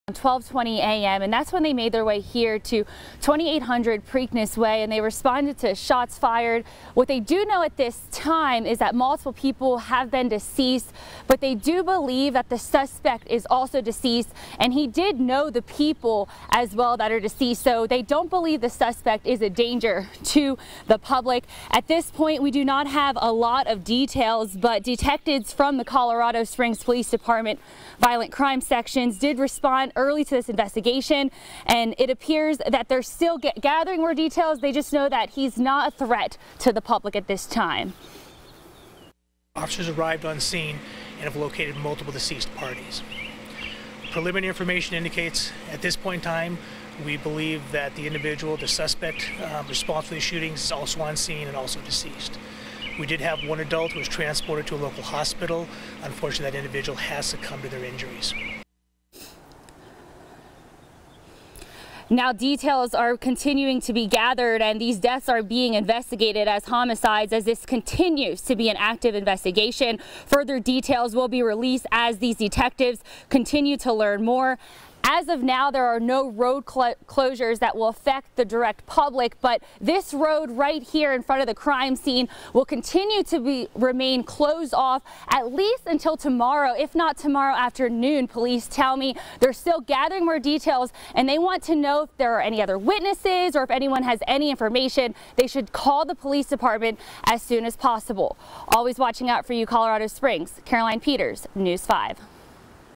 Colorado Springs Police: Multiple deceased victims found inside Preakness Way home
According to police, a call came in just after midnight for a shooting inside the Canterbury Manufactured Home Community.   Officers found multiple people dead in the area, without giving an exact number of people who have died. They do believe the suspect and victims knew each other.